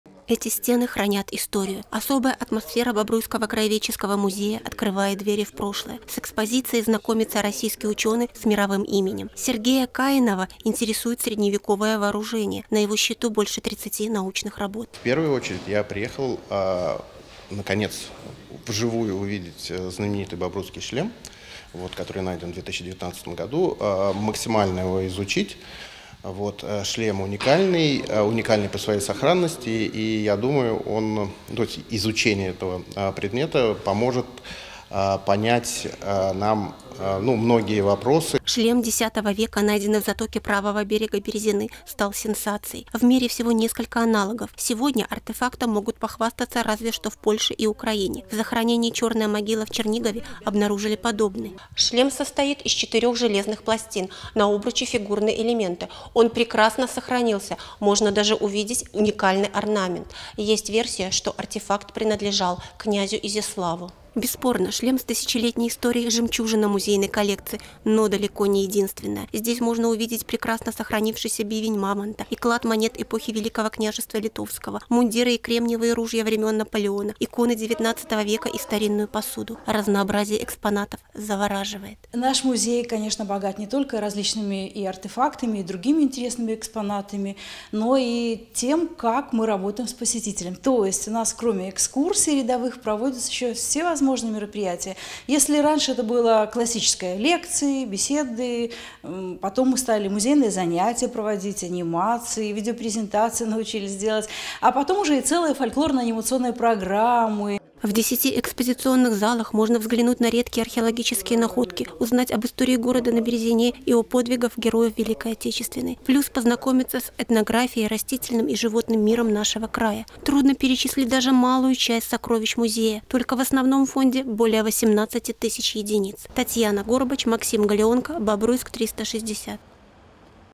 Через призму времени
Бобруйский  артефакт с тысячелетней историей заинтересовал  российских учёных.